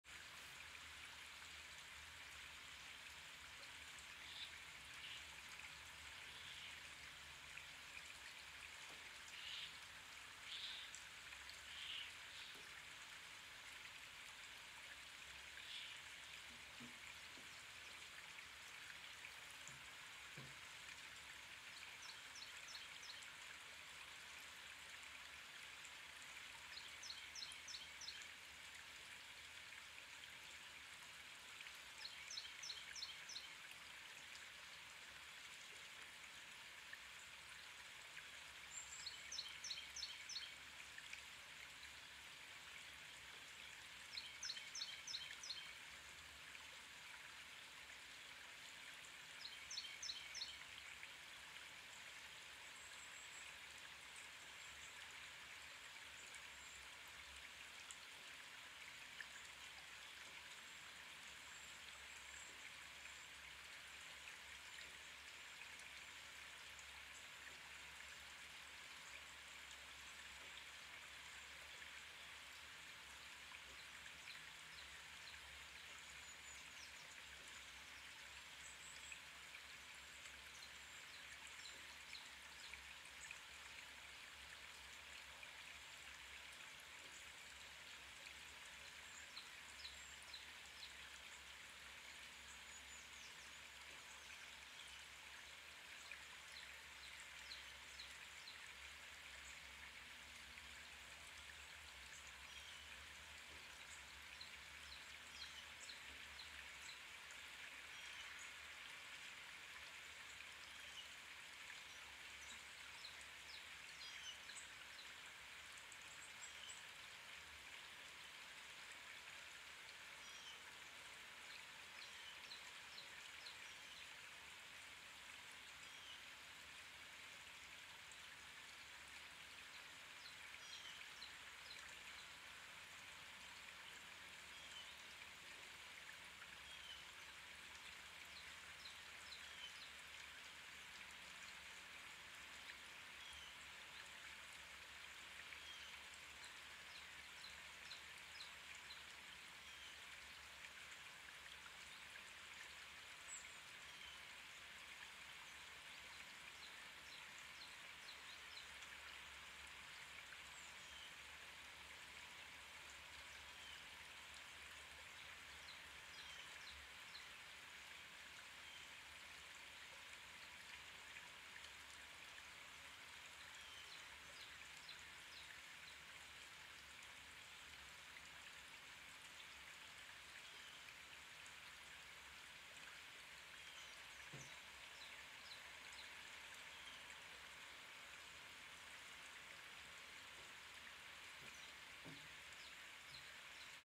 HERBSTKRANZ MIT NATURMATERIALIEN AUS DEM GARTEN 🍃 DIY HERBSTDEKO 🍁
Herbstkranz basteln, einfach aus dem eigenen Garten…